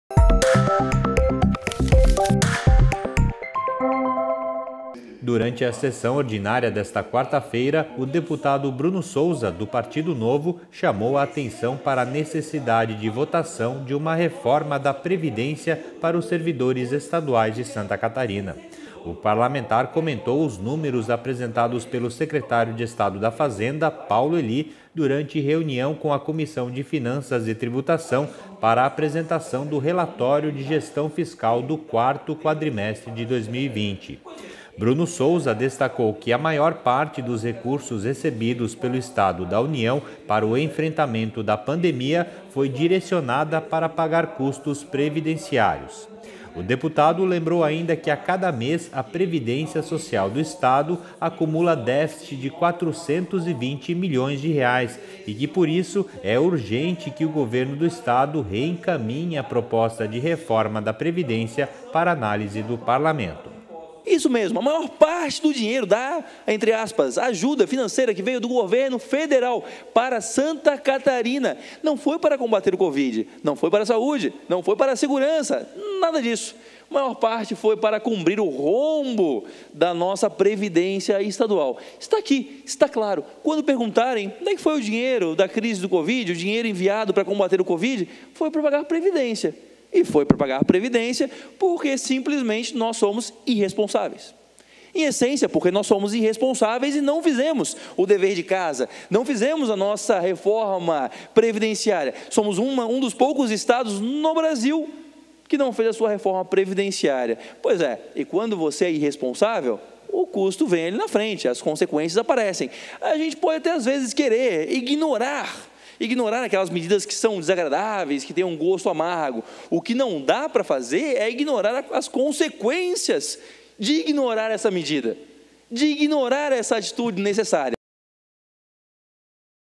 É Notícia - Deputado cobra do Executivo envio de proposta da Reforma da Previdência - 26/05/21
Durante a sessão ordinária desta quarta-feira (26), o deputado Bruno Souza (Novo) chamou a atenção para a necessidade de votação de uma reforma da previdência para os servidores estaduais em Santa Catarina.